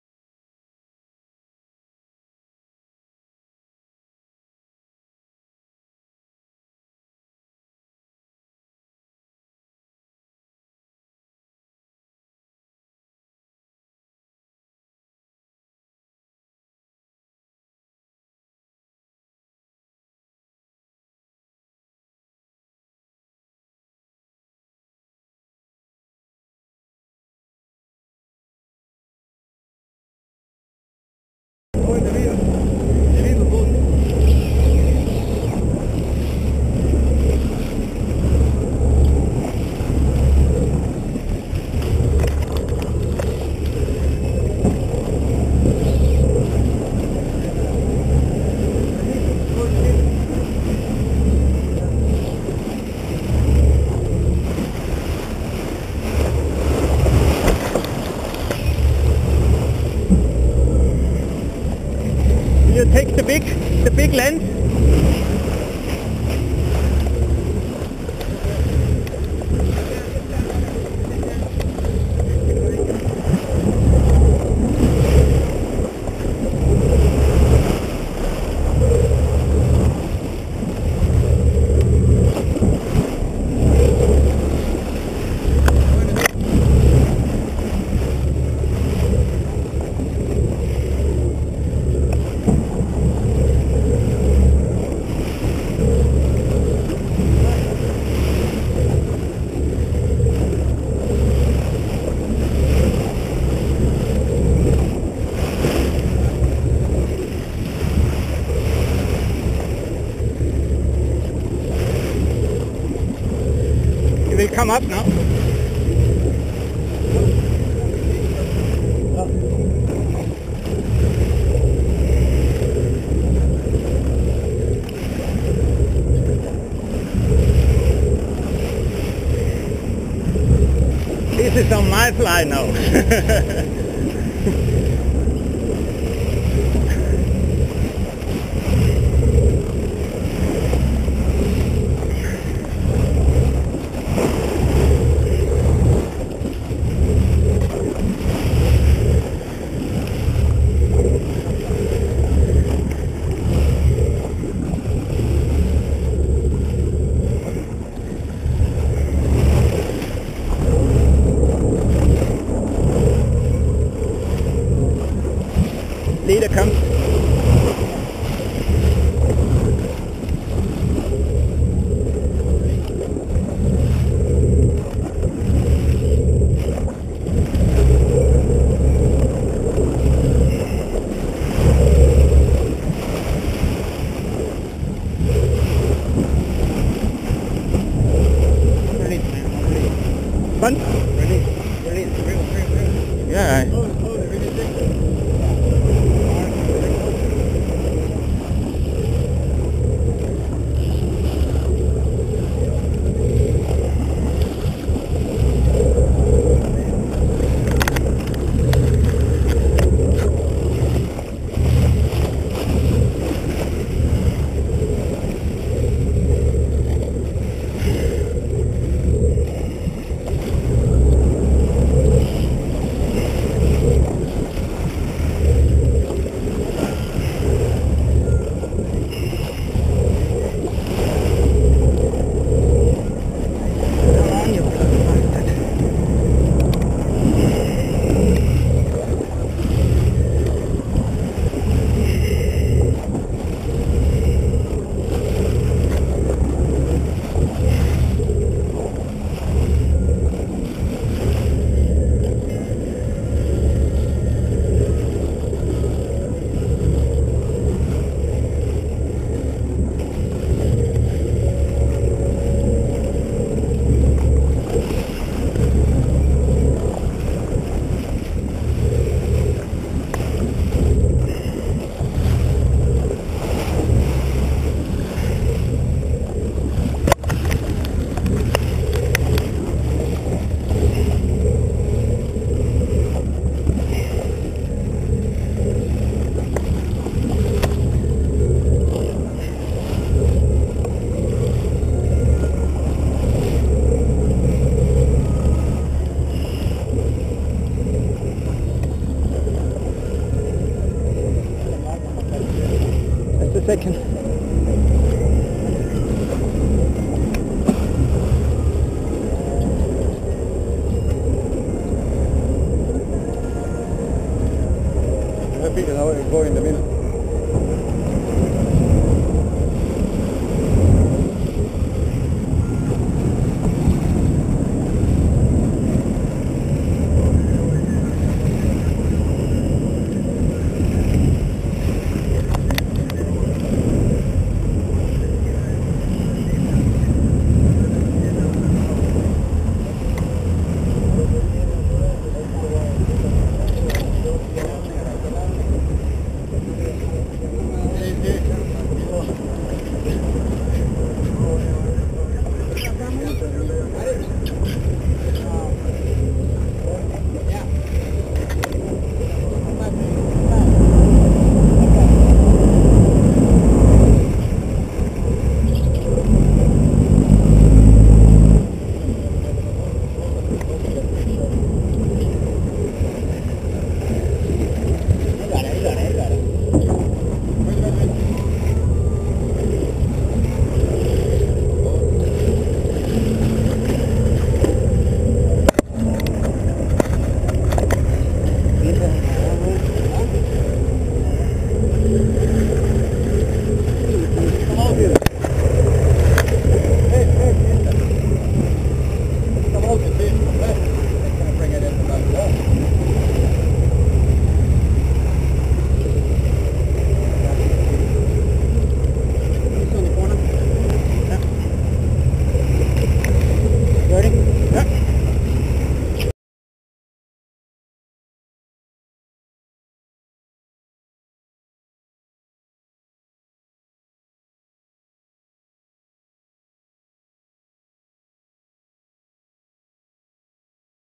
Sailfish Power versus tackle power by Guenter Feuerstein
How long does it take to fight a 50 kg sailfish with a fly rod? Watch a tackle testing fight in real time. Don't copy without protectors!